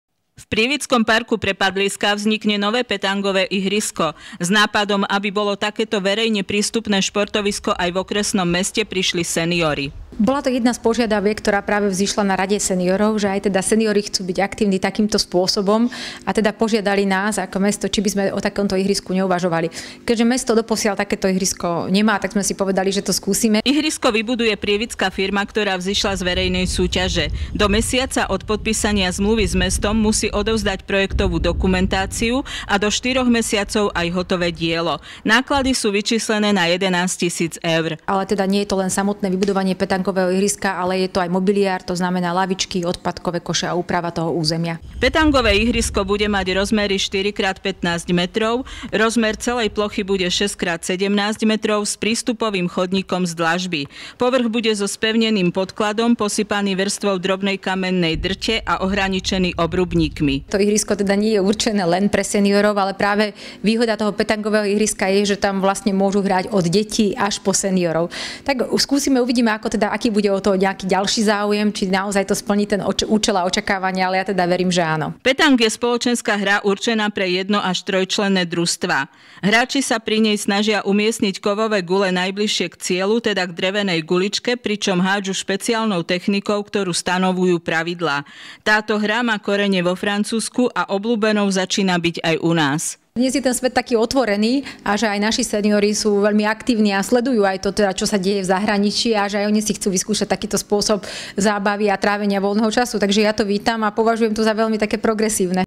Na Prepadliskách bude petangové ihrisko
Petang začína byť stále populárnejšou športovou hrou aj v našich končinách. Petangové ihrisko bude mať aj Prievidza.  Miesto na jeho zriadenie vybrala samospráva v parku Prepadliská.